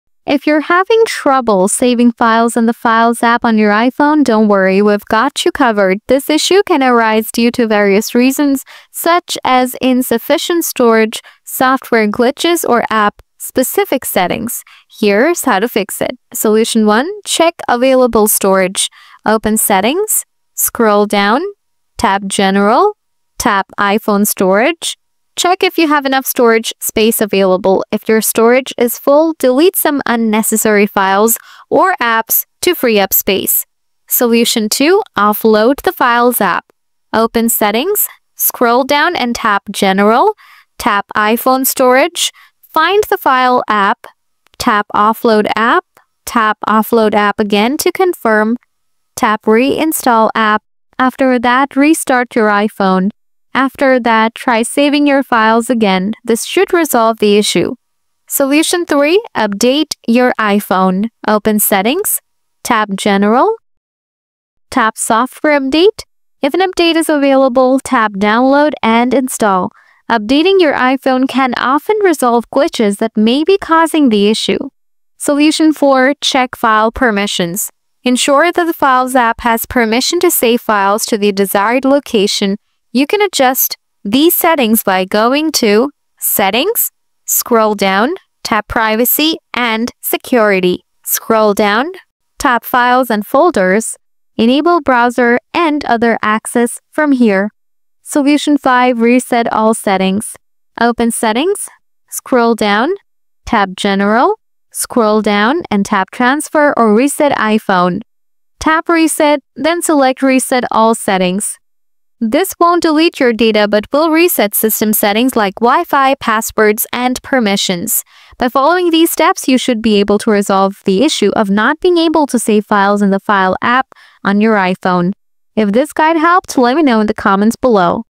How To Fix Can’t Save Files in File App on iPhone | File app issues on IOS
Struggling to save files in the Files app on your iPhone? This issue may occur due to insufficient storage, incorrect permissions, or outdated iOS software. In this guide, learn how to fix the "Can't Save Files" problem by clearing storage, adjusting app permissions, and ensuring your iPhone is updated to the latest iOS version. Follow these steps to regain full functionality and easily save your files without any hassle!